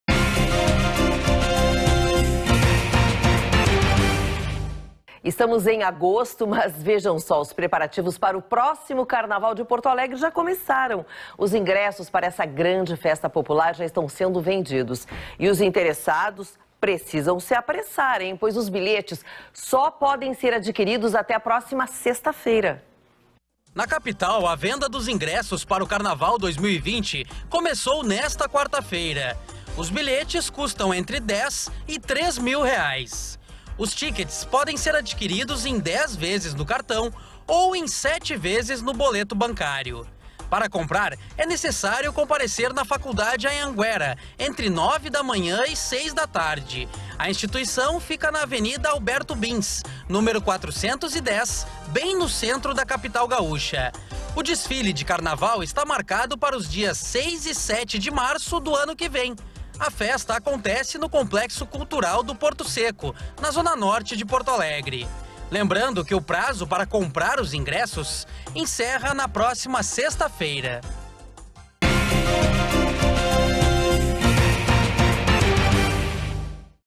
Ingressos para o Carnaval de Porto Alegre já podem ser adquiridos | Jornal da Pampa | 21/08/2019
Ingressos para o Carnaval de Porto Alegre já podem ser adquiridos.